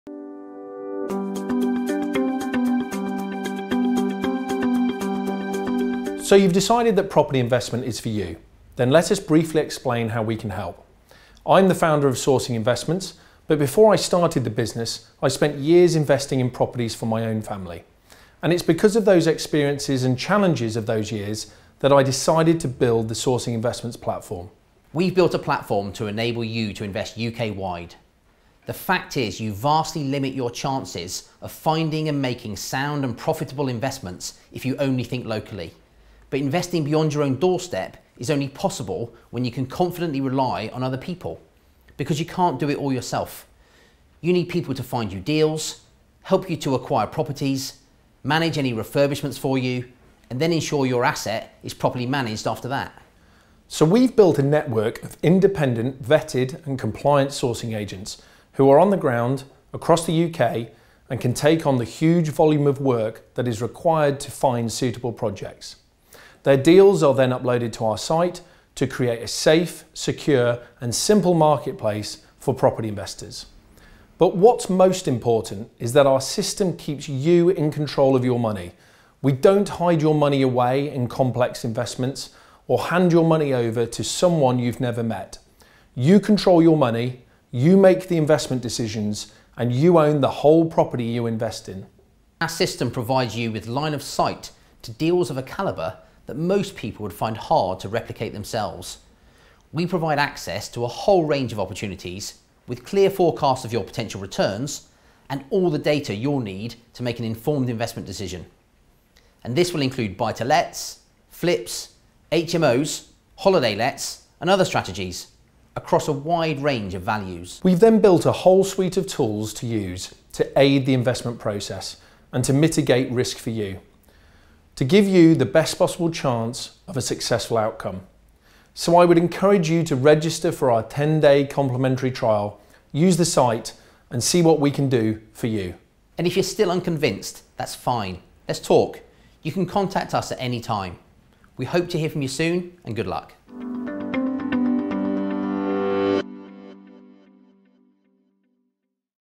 Who are Sourcing Investments?
𝐖𝐞𝐥𝐜𝐨𝐦𝐞 𝐭𝐨 𝐭𝐡𝐞 𝐒𝐨𝐮𝐫𝐜𝐢𝐧𝐠 𝐈𝐧𝐯𝐞𝐬𝐭𝐦𝐞𝐧𝐭𝐬 𝐏𝐫𝐨𝐩𝐞𝐫𝐭𝐲 𝐂𝐡𝐚𝐧𝐧𝐞𝐥

A Complete Property Investment Solution for worldwide investors and a complete digital platform for sourcers to efficiently manage your sourcing business and maximise your revenue.

Find out more about our unique property platform that brings together Verified Property Investors and Pre-Vetted Sourcers for amazing deals that you simply won´t find anywhere else.